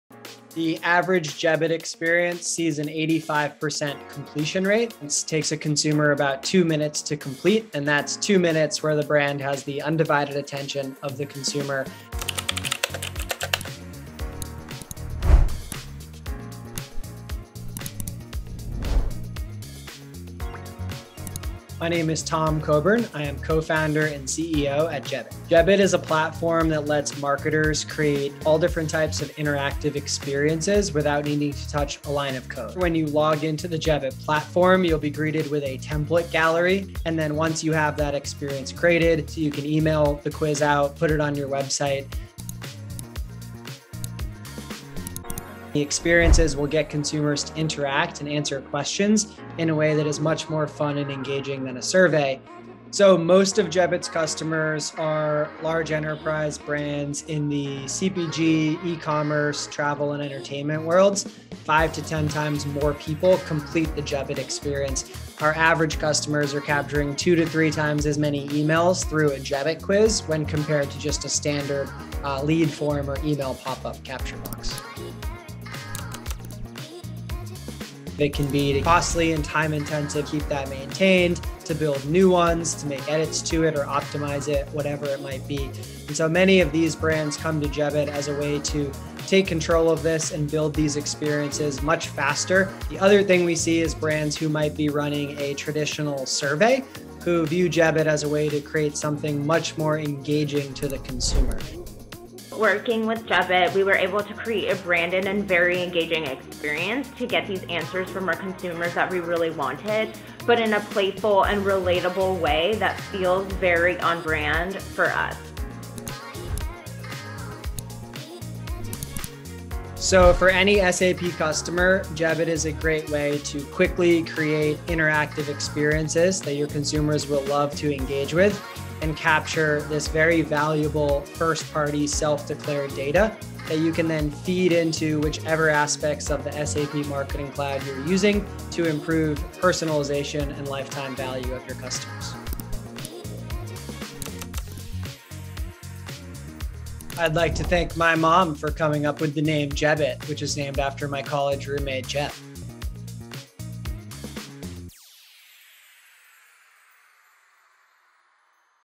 Create engaging and interactive experiences that your consumers will love with Jebbit
Meet Tom Coburn, Co-Founder & CEO of Jebbit. Jebbit is a no-code platform that allows companies to capture valuable 1st party declared data (also referred to as zero party data) by creating highly engaging interactive experiences

Jebbit is a graduate of SAP.iO Foundry Tel Aviv: Consumer Engagement (2021) Cohort.